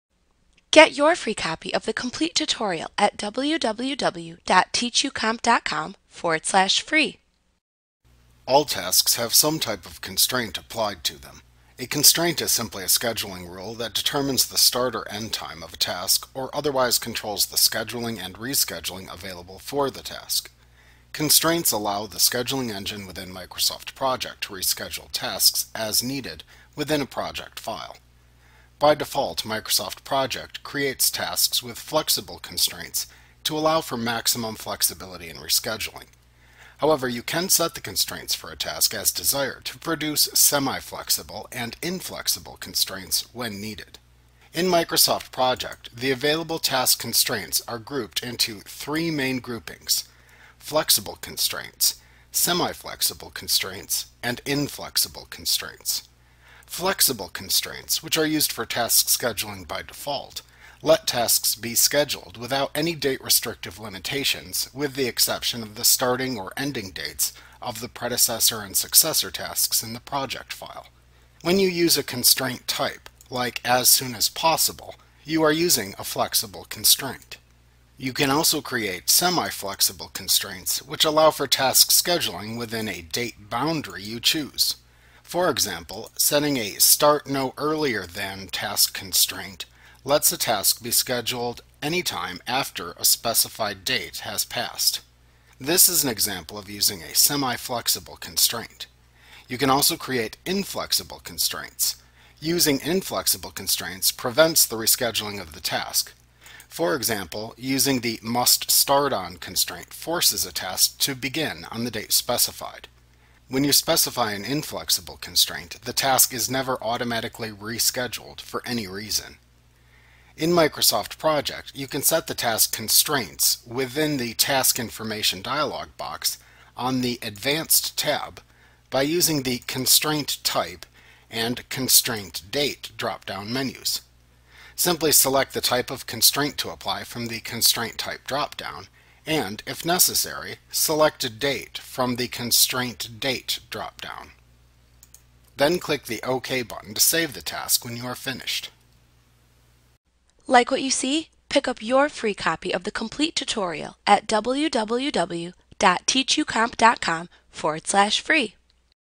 Project 2016 Tutorial Using Task Constraints Microsoft Training
Learn about using task constraints in Microsoft Project A clip from Mastering Project Made Easy v. 2016. - the most comprehensive Project tutorial available. Visit us today!